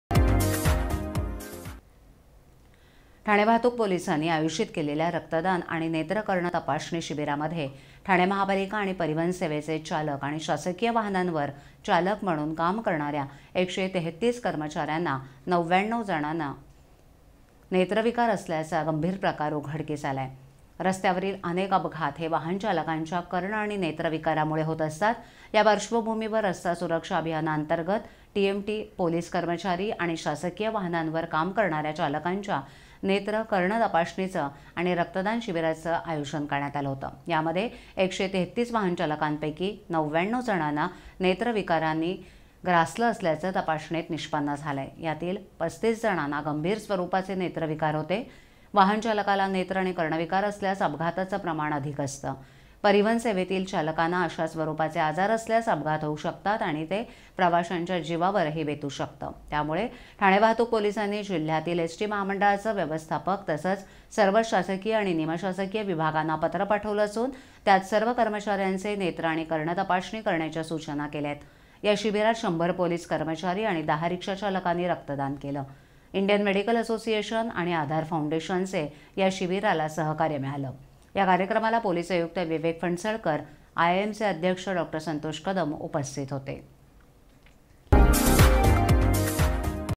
Aawaz maza
टीएमटी चालकांमध्ये आढळलं नेत्रविकाराचे प्रमाण जास्त.